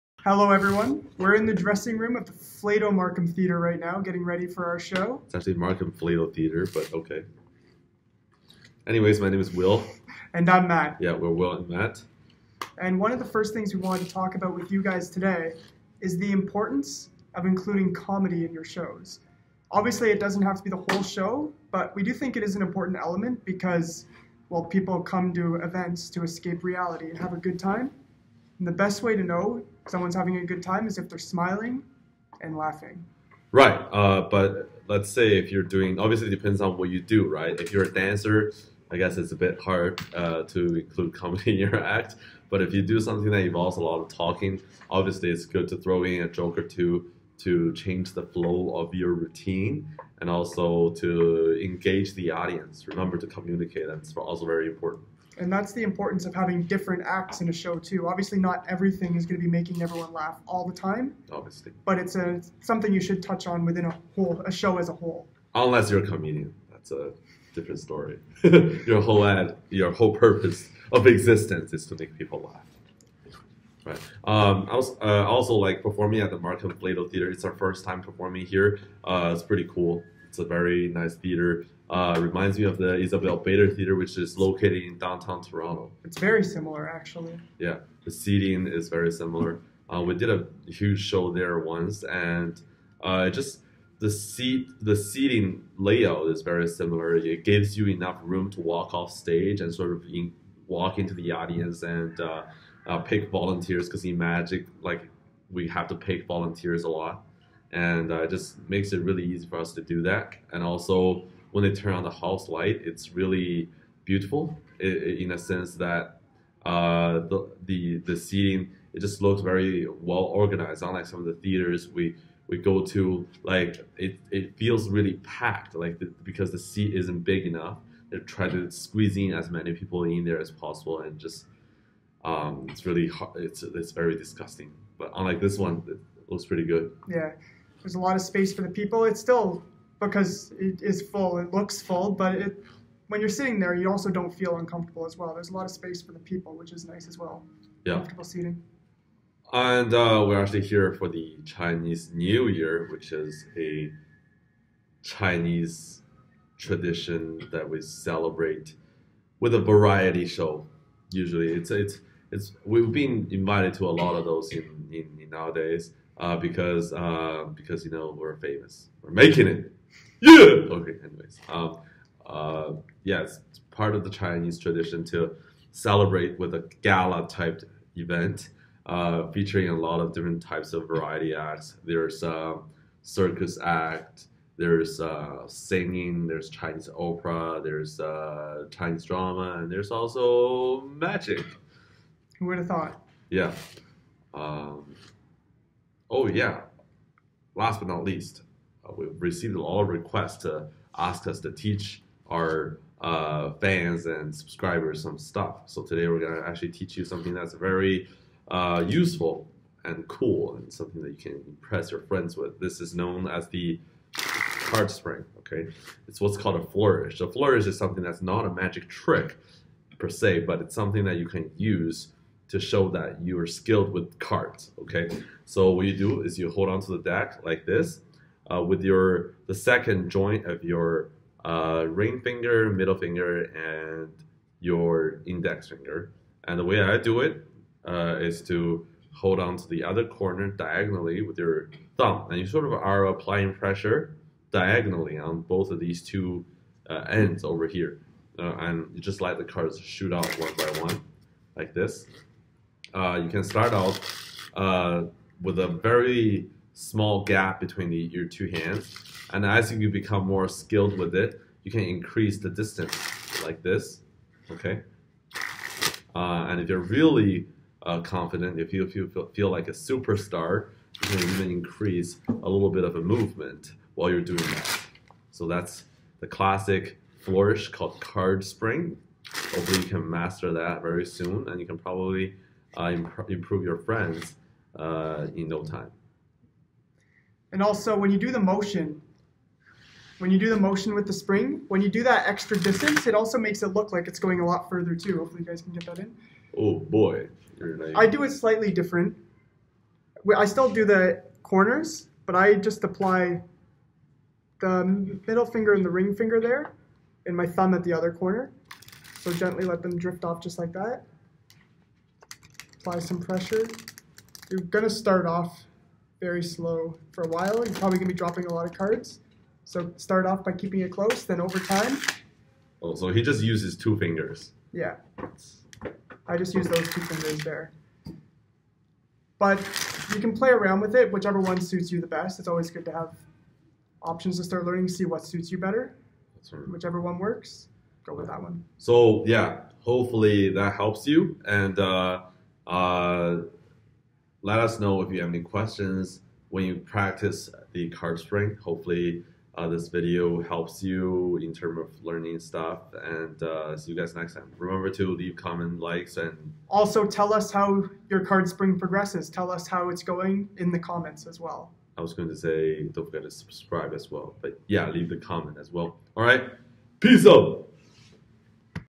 Comedy, Chinese new year, and Card sleight of hand tutorial
Right before we are about to go on stage at the flato markham theater for the Chinese new year gala, we decided to shoot this video. At the end of the video we will also teach a card leight of hand move